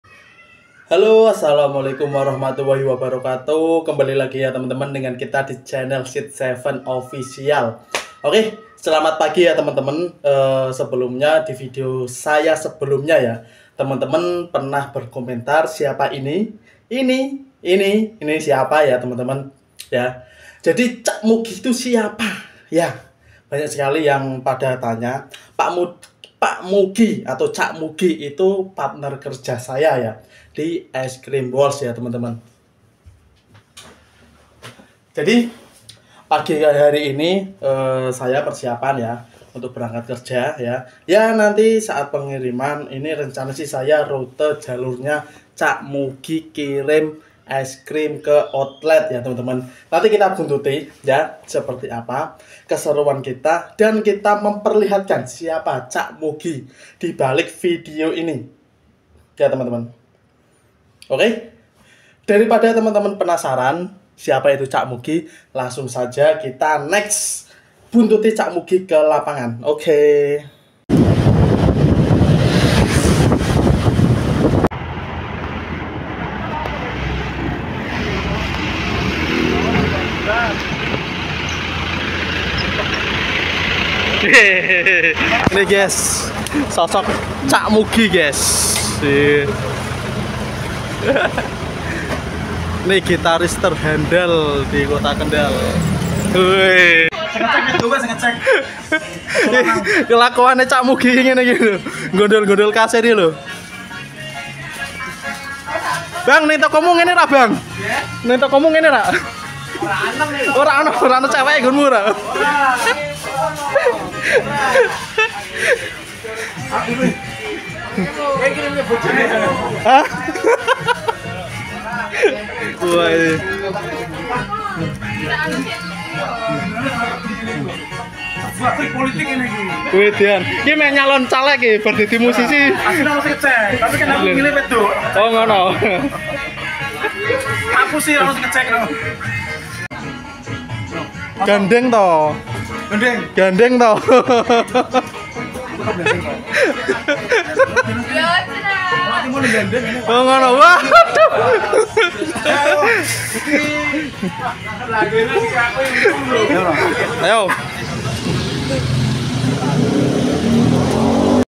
MENGENAL SOSOK CAK MUGI GITARIS AMBYAR
Hallo Gaes kali ini saya lihatkan ya sosok cak mugi, kenapa saya sering post vidio dia

dan vidio kali ini saya ambil atas izin semua pihak ya. tdk ada unsur di luar norma, karena semua bercanda dan sudah di setting

terimakasih